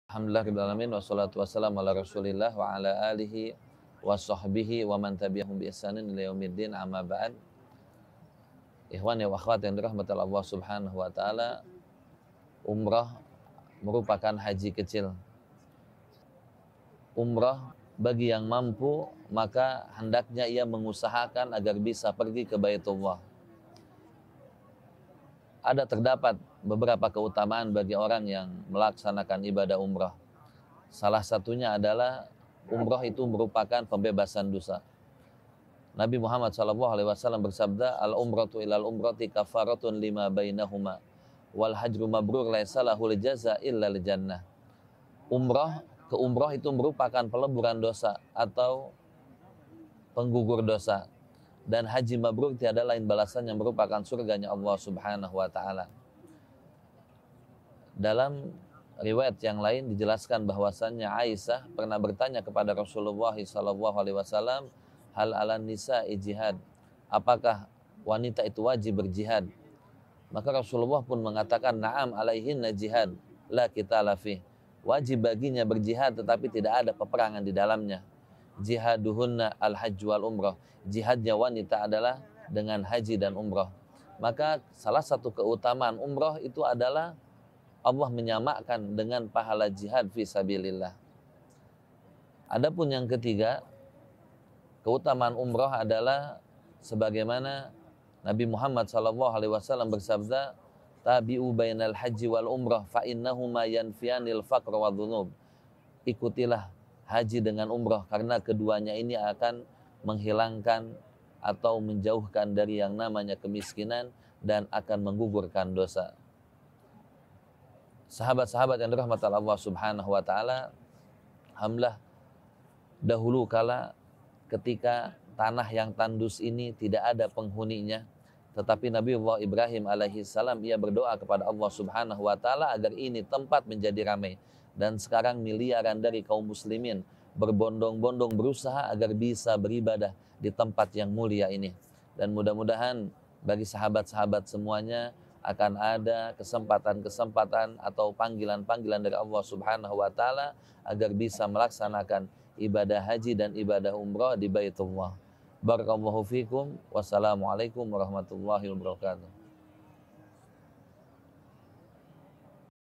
Inilah Keutamaan Umroh Yang Harusnya Kita Tau - Ustadz Helmi Khalid Syammach
Setiap orang pasti merindukan tanah suci, apalagi Ka’bah di tanah haram Makkah. Di tempat tersebut ada dua aktivitas ibadah yang mulia yaitu umrah dan haji. Untuk umrah sendiri bisa dilakukan setiap saat. Sedangkan haji hanya khusus di bulan haji, bulan Dzulhijjah.

Apa saja keutamaan ibadah umrah? Simak penjelasan Ustadz Helmi Khalid Syammach.

▶️  Informasi & Pendaftaran Umroh:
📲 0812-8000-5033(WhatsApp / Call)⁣
✈️ PT. Masirah Madinah Thayibah